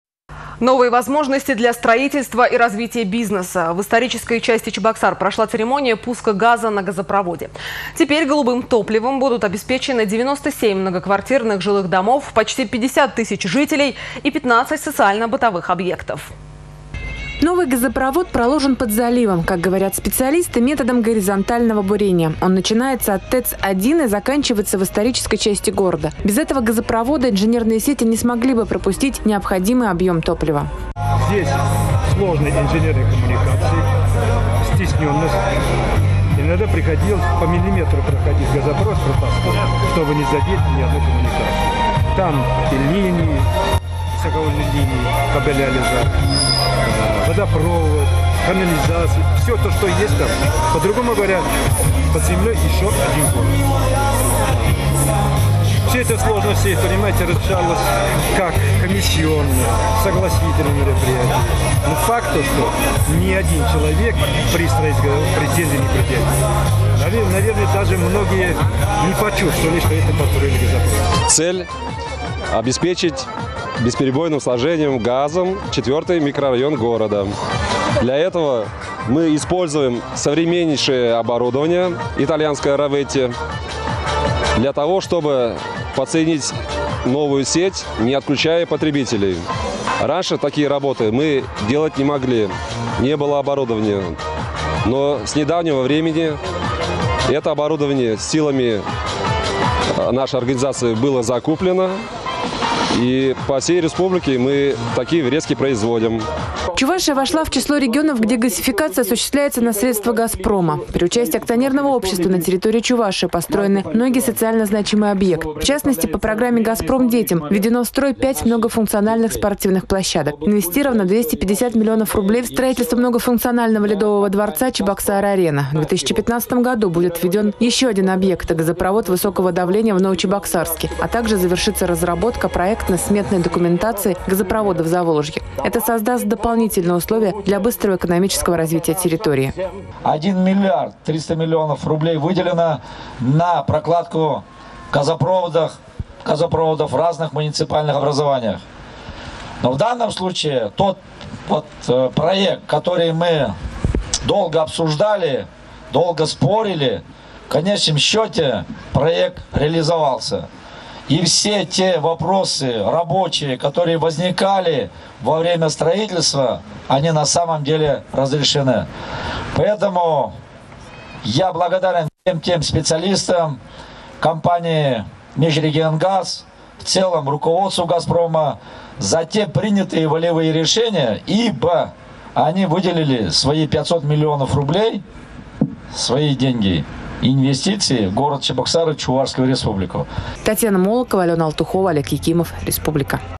Церемония пуска газа на газопроводе
Новые возможности для строительства и развития бизнеса — в исторической части Чебоксар прошла церемония пуска газа на газопроводе. Теперь голубым топливом будут обеспечены 97 многоквартирных жилых домов, почти 50 тысяч жителей и 15 социально-бытовых объектов.